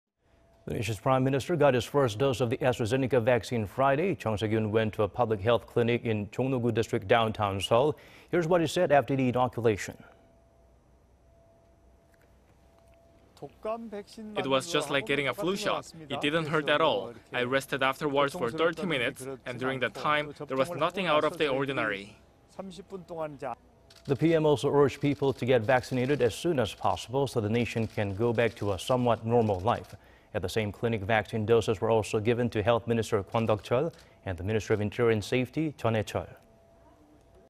S. Korean PM gets first dose of COVID-19 vaccine
아스트라 백신 접종한 정총리... '하나도 안 아파... 독감 백신과 똑같아'

The nation's Prime Minister got his first dose of the AstraZeneca vaccine Friday.
Chung Sye-kyun went to a public health clinic in the Jongno-gu District, downtown Seoul.
Here's what he said after the inoculation.

"It was just like getting a flu shot. It didn't hurt at all. I rested afterwards for 30 minutes, and during that time, there was nothing out of the ordinary."

The PM also urged people to get vaccinated as soon as possible so the nation can go back to a somewhat normal life.
At the same clinic, vaccine doses were also given to Health Minister  Kwon Deok-cheol... and the Minister of Interior and Safety,  Jeon Hae-cheol.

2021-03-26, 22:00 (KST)